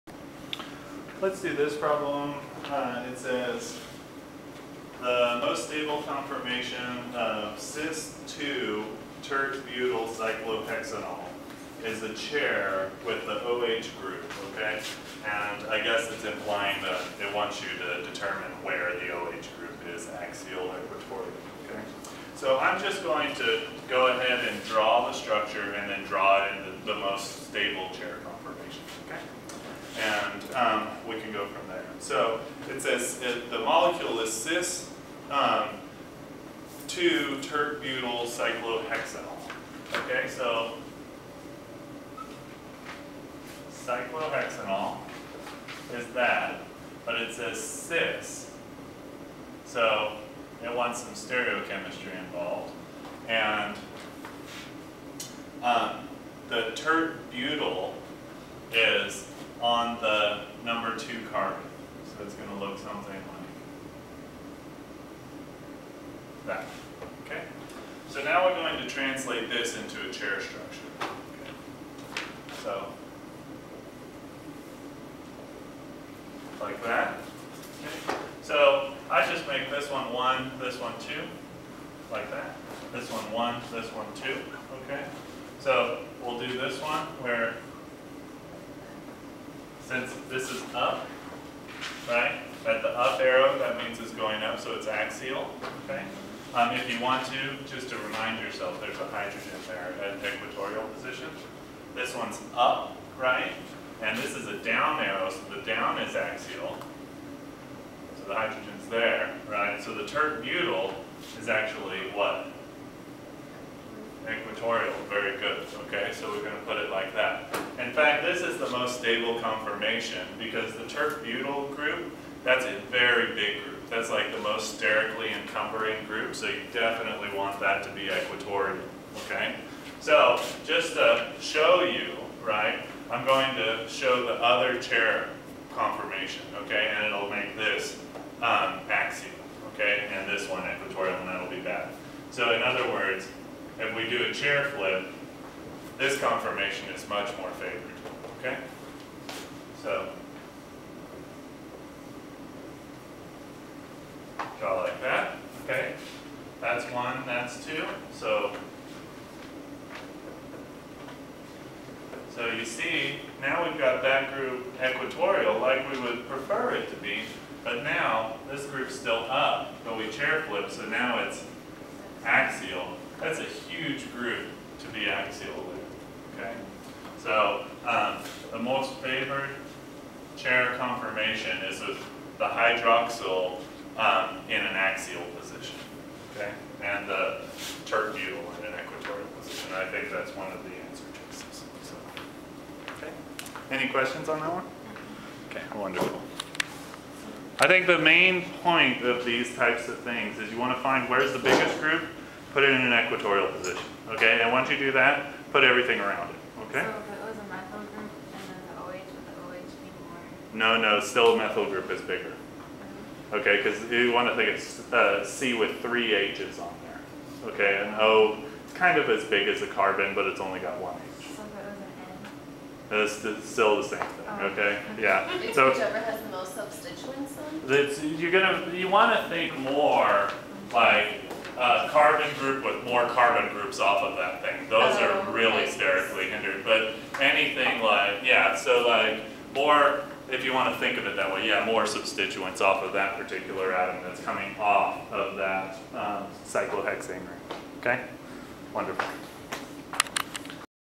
Cyclohexane Ring Systems - Draw most stable conformer 009
Draw the most stable chair conformer of cis-2-tert-butylcyclohexanol.